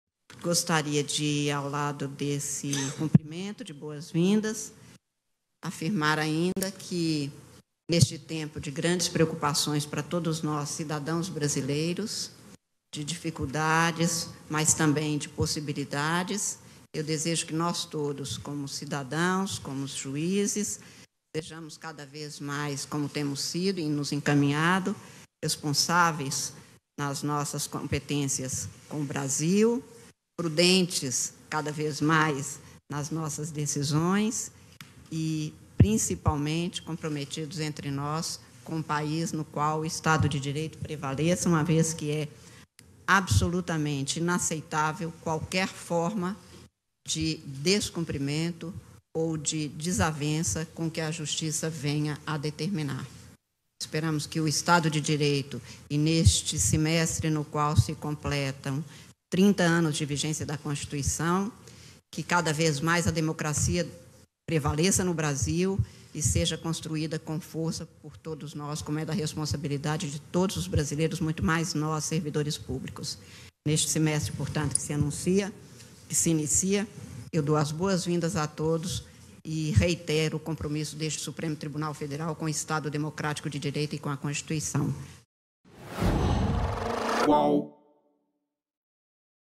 CÁRMEN LÚCIA PEDE PRUDÊNCIA EM "TEMPO DE GRANDES PREOCUPAÇÕES"
Na sessão em que retoma os trabalhos do STF (Supremo Tribunal Federal) após o recesso de julho, a presidente da Corte, ministra Cármen Lúcia, pediu prudência a cidadãos e juízes “nesse tempo de grandes preocupações para nós cidadãos brasileiros, de dificuldades, mas também de possibilidades”. “Eu desejo que nós todos, como cidadãos, como juízes, sejamos cada vez mais, como tem sido e encaminhado, responsáveis com as nossas competências com o Brasil, prudentes cada vez mais nas nossas decisões e, principalmente, comprometidos entre nós com um país no qual o Estado de Direito prevaleça”, disse a ministra em seu discurso na abertura da sessão desta quarta-feira (1º).